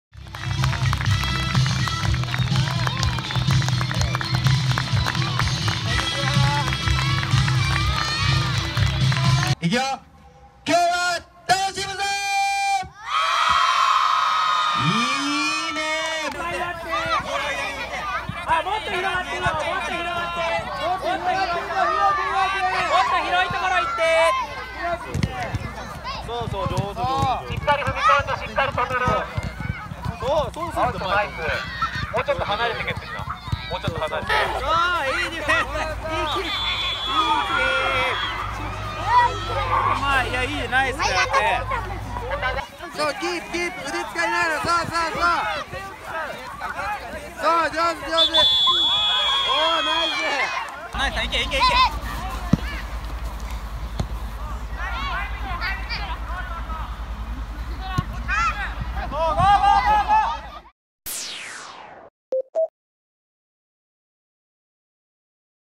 真備で被災地励ますサッカー教室（2019/1/13）
西日本豪雨の被災地に元気を―と、「三菱自動車＆浦和レッズ　サッカーフェスティバル」が１３日、倉敷市真備町箭田の真備総合公園野球場で開かれた。Ｊ１浦和レッズから柏木陽介、宇賀神友弥、森脇良太の元日本代表３選手らが訪れ、子どもたちを激励した。